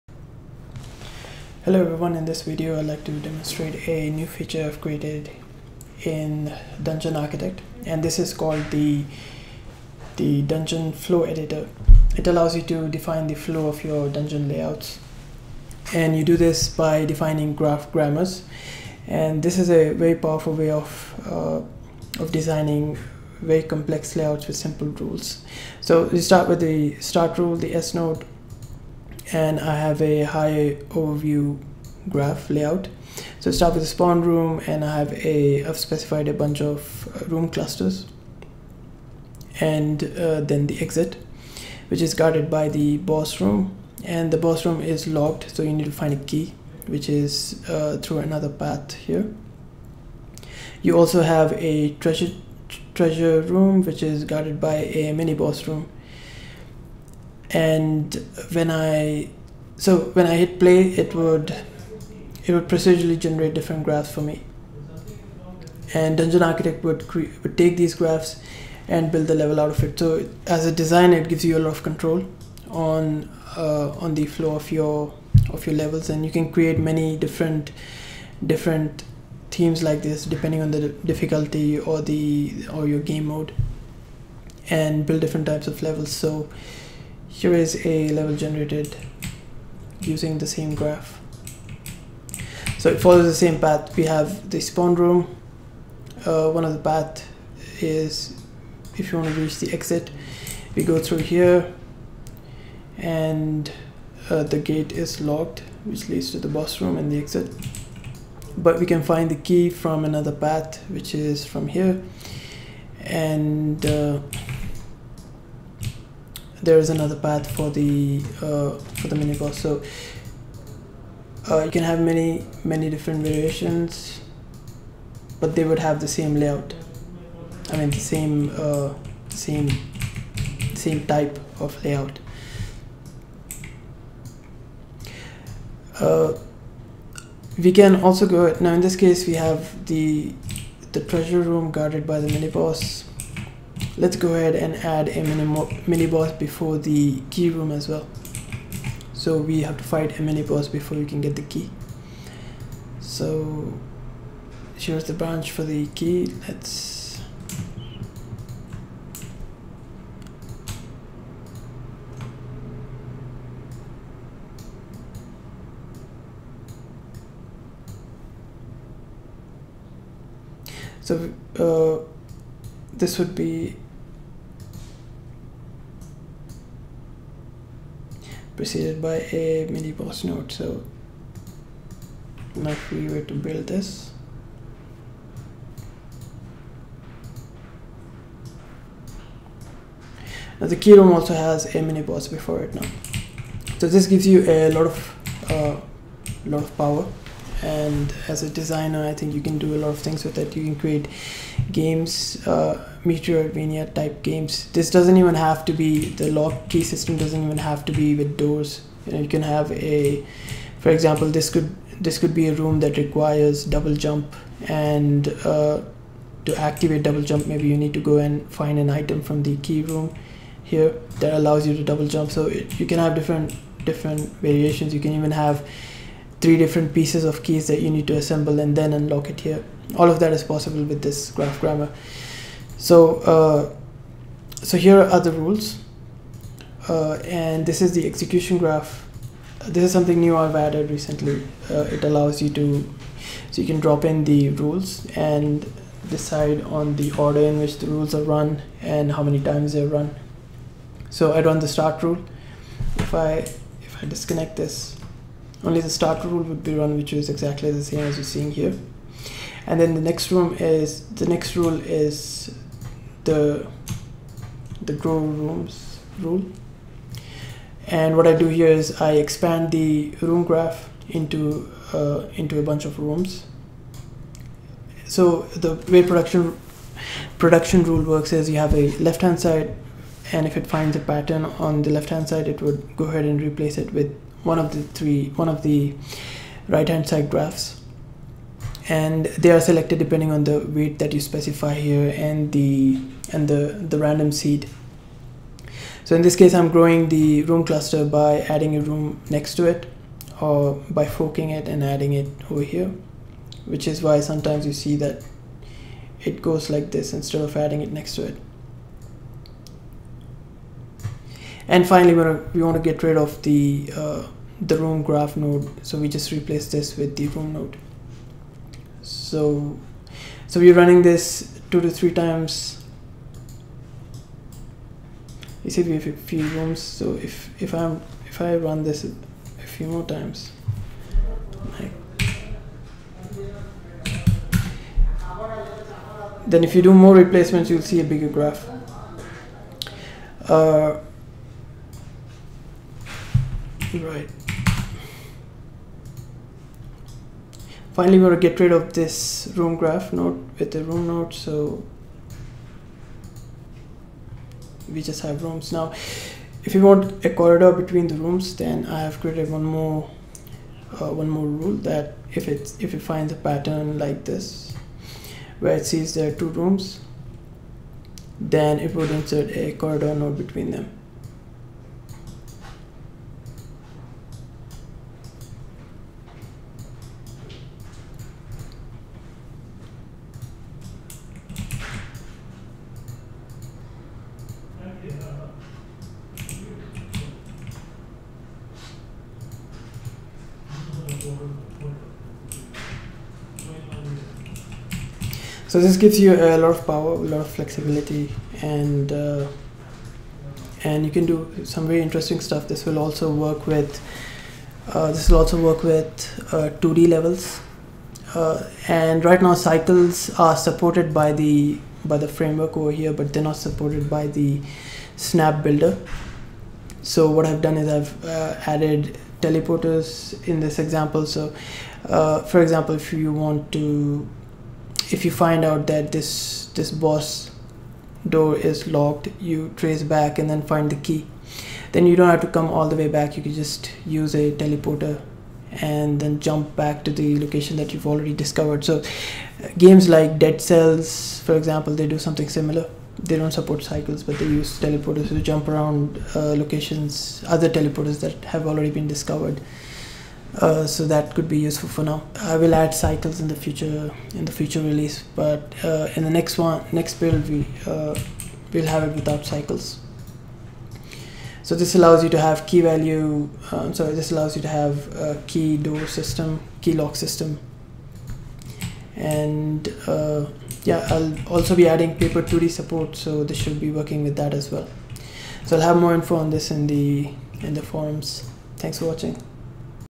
Dungeon Architect: Flow Editor with Snap Builder
A powerful new addition to Dungeon Architect lets you use Graph Grammars to generate layouts for your dungeons